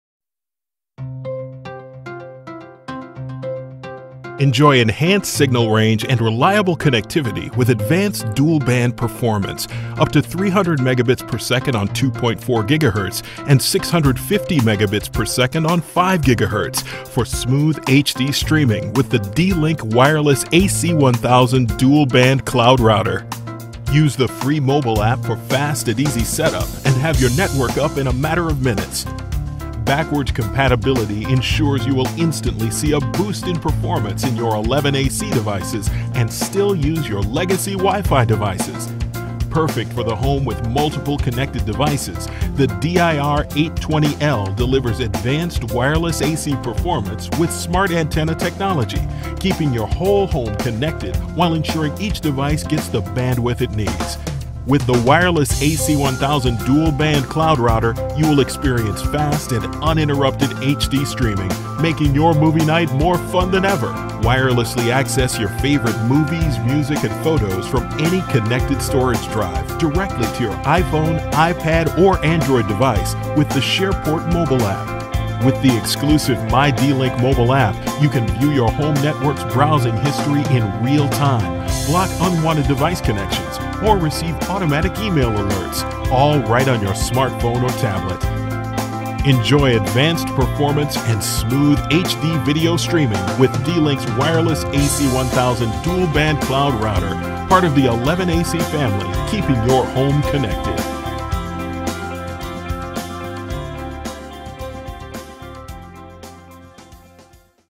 D-Link - DIR-820L Wireless AC1000 Dual Band Cloud Router
Il D-Link ® Wireless AC1000 dual Router Nube Band (DIR-820L) offre connettività affidabile con la prossima generazione di tecnologia 802.11ac e velocità wireless avanzate prestazioni fino a 1000 Mbps, 300 Mbps (2,4 GHz) e 650Mbps (5,0 GHz) per complesse reti domestiche deve .